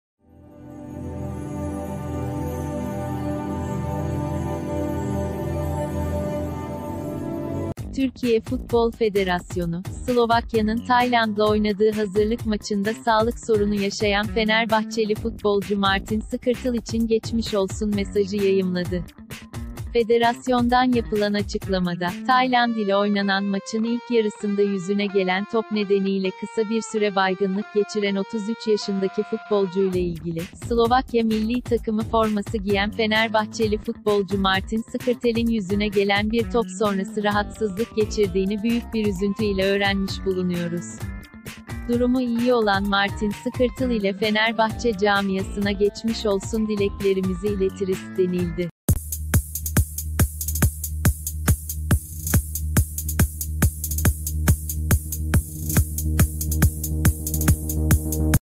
Son Dakika Haberleri | TFF'den Skrtel'e "geçmiş olsun" mesajı
Türkiye Futbol Federasyonu, Slovakya'nın Tayland'la oynadığı hazırlık maçında sağlık sorunu yaşayan Fenerbahçeli futbolcu Martin Skrtel için geçmiş olsun mesajı yayımladı. Federasyondan yapılan açıklamada, Tayland ile oynanan maçın ilk yarısında yüzüne gelen top nedeniyle kısa bir süre baygınlık geçiren 33 yaşındaki futbolcuyla ilgili, "Slovakya Milli Takımı forması giyen Fenerbahçeli futbolcu Martin Skrtel'in yüzüne gelen bir top sonrası rahatsızlık geçirdiğini büyük bir üzüntü ile öğrenmiş bul...